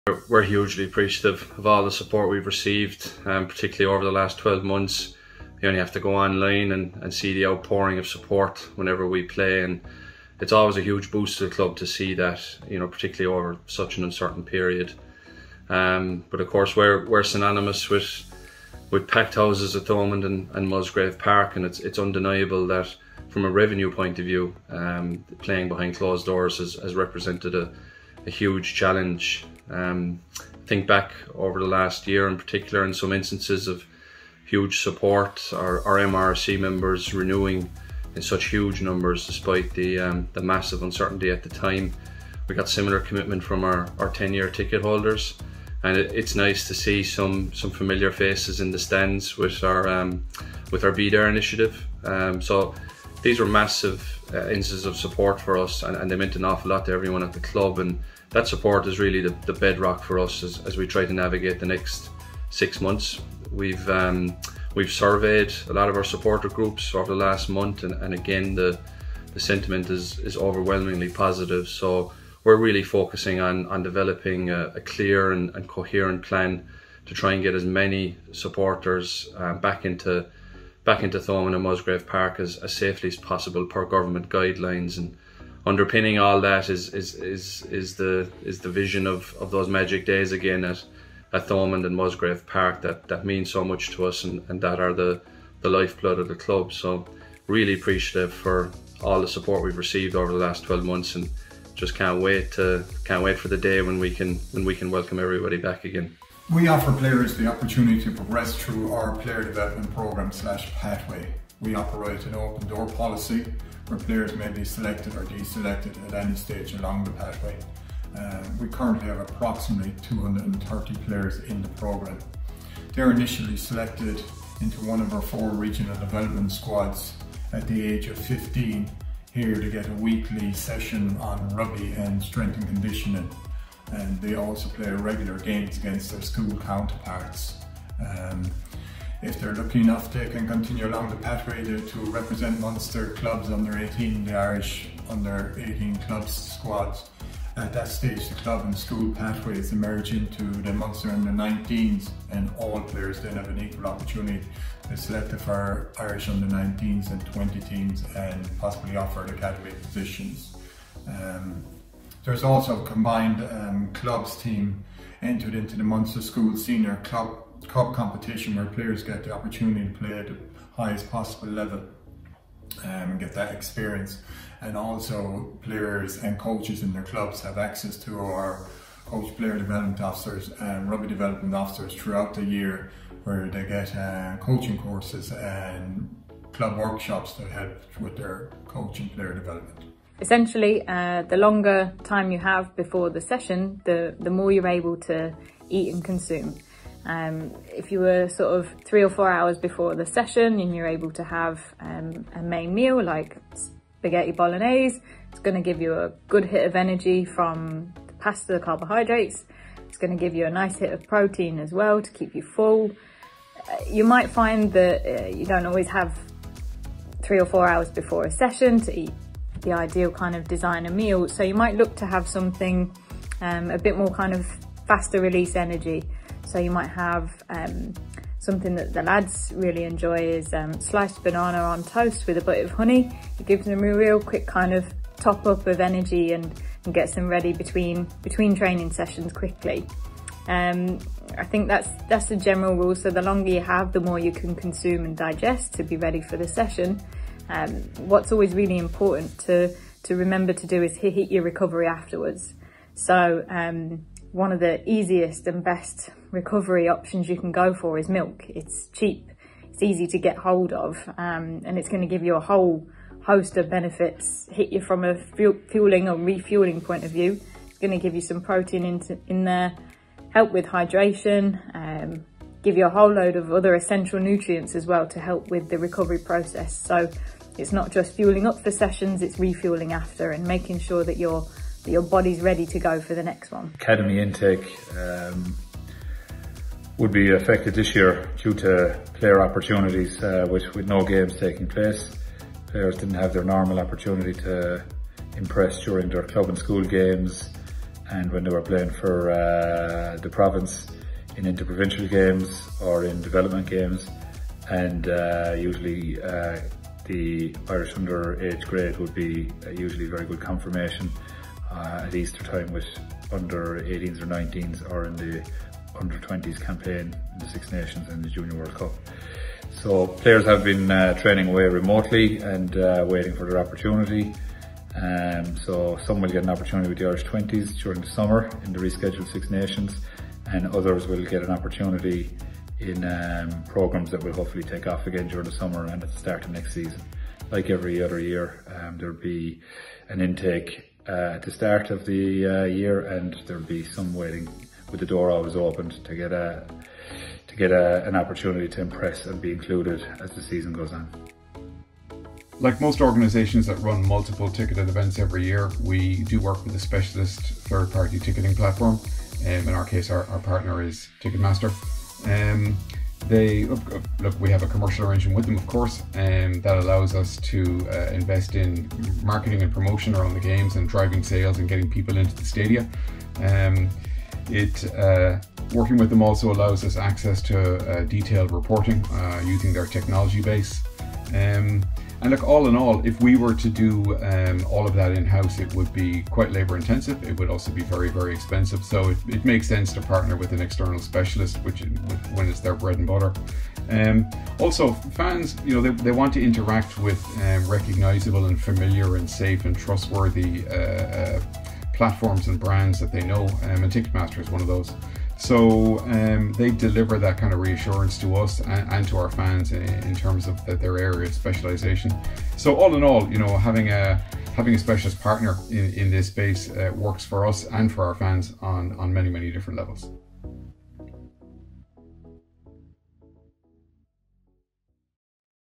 State of Play | April 2021
State of Play is our fan-led series where you set the agenda with Munster Rugby management and staff answering your questions on all aspects of rugby in the province.